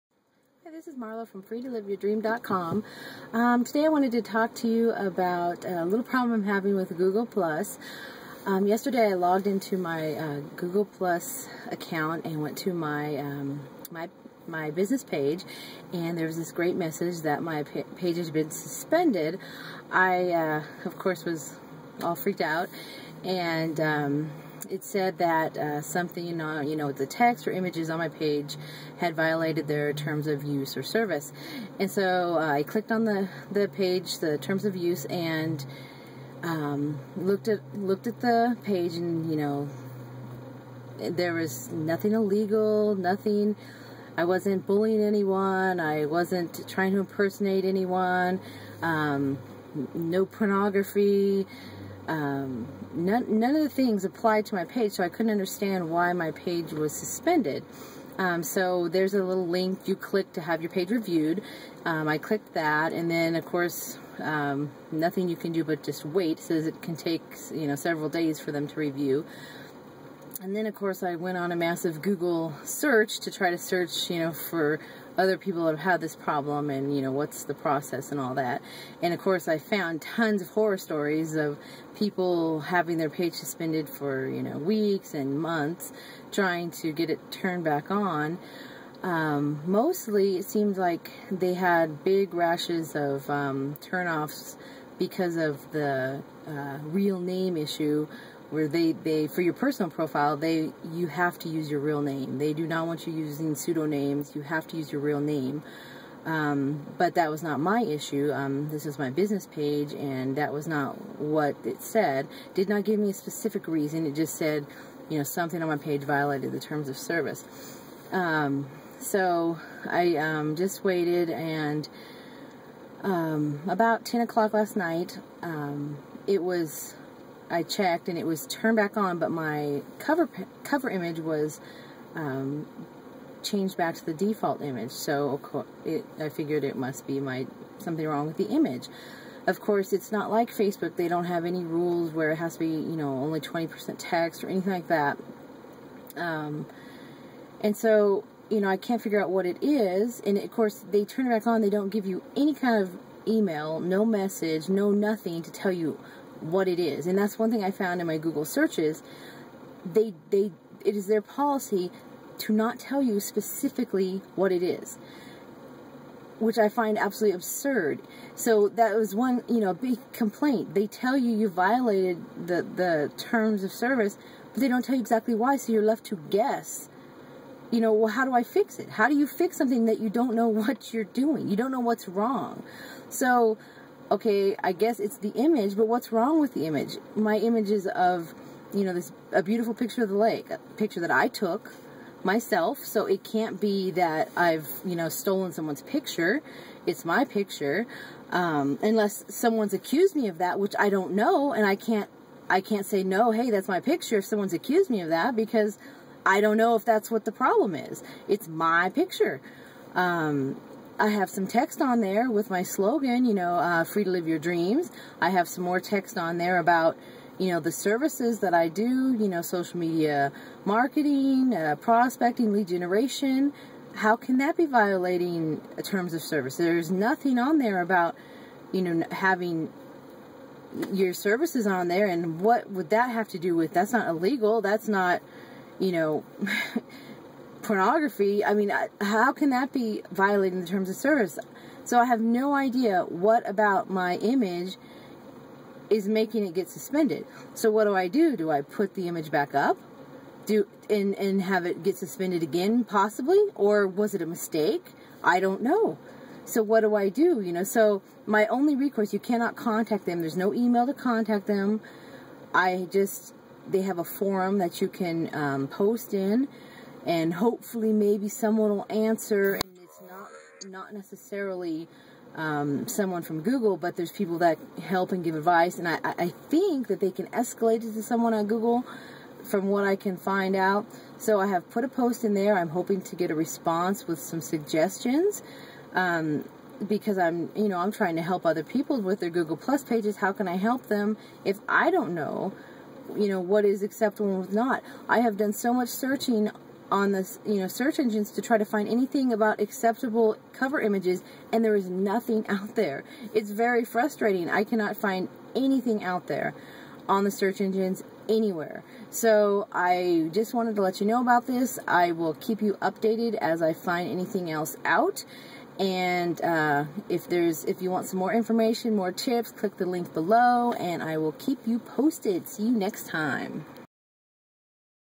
Google+ Tips | My Google Business Page Was Suspended
- My Google+ business page was suspended for cover image violating their TOS/TOU. Not sure why or how. In this video I describe the process and steps involved and my frustration.